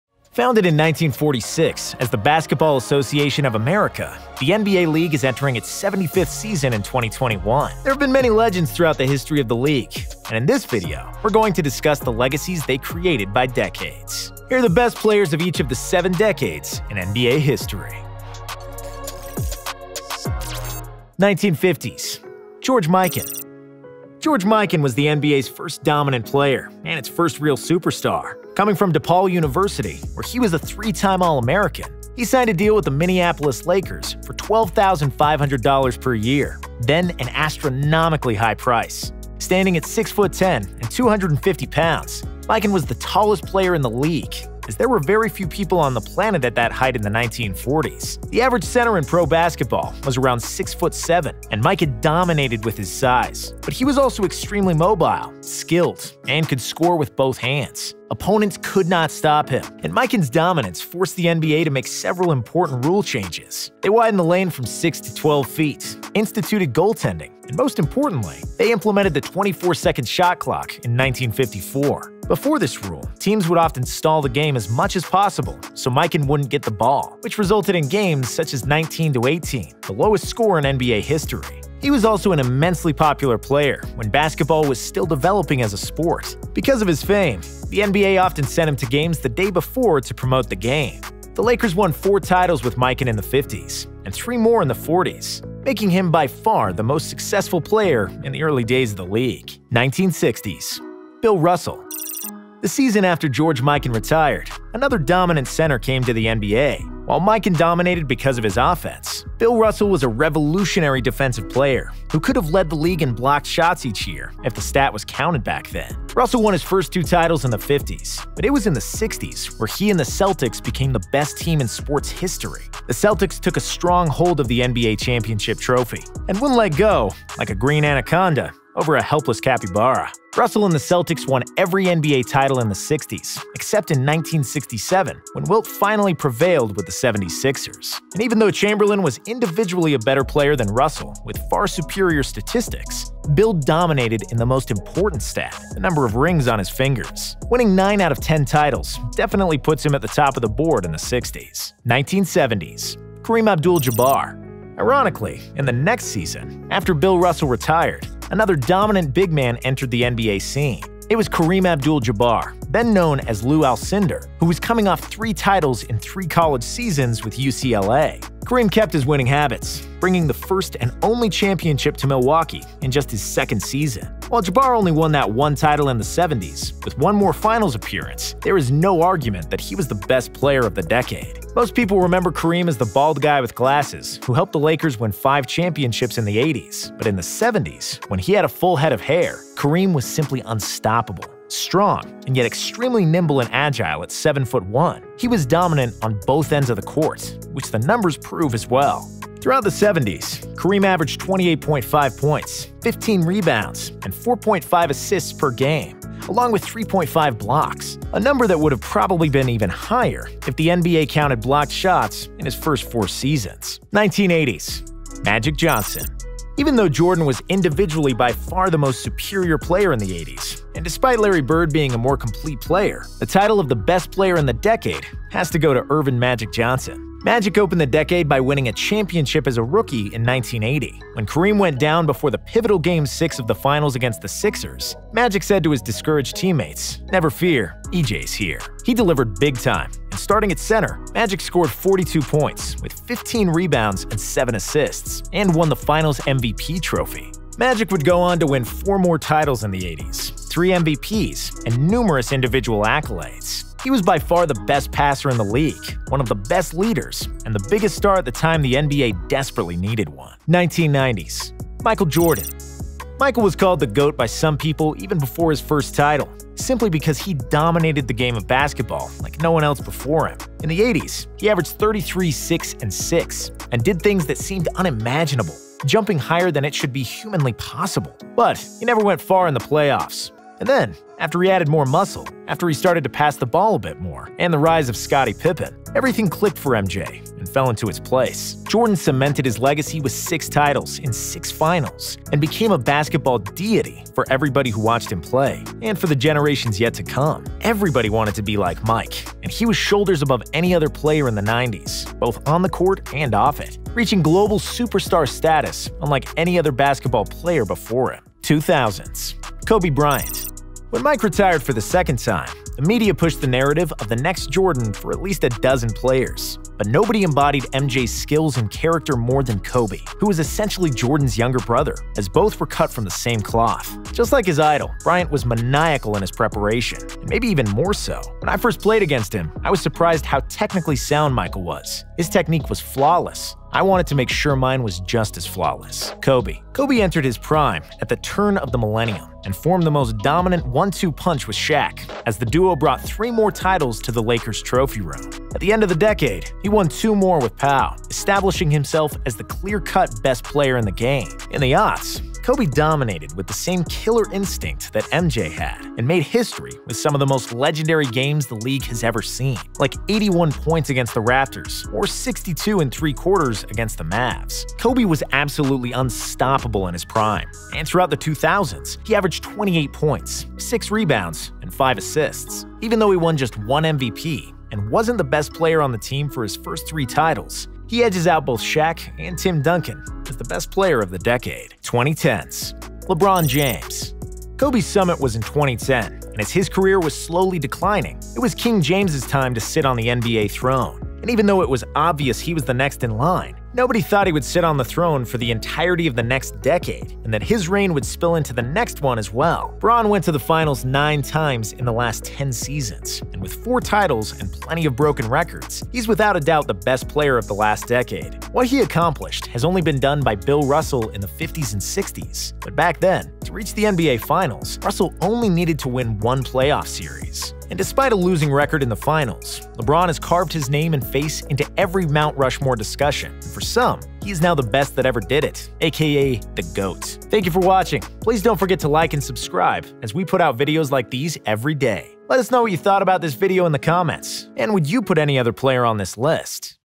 Best Player From Every Decade In NBA History
There have been many legends throughout the history of the league, and in this video, we’re going to discuss the legacies they created by decades.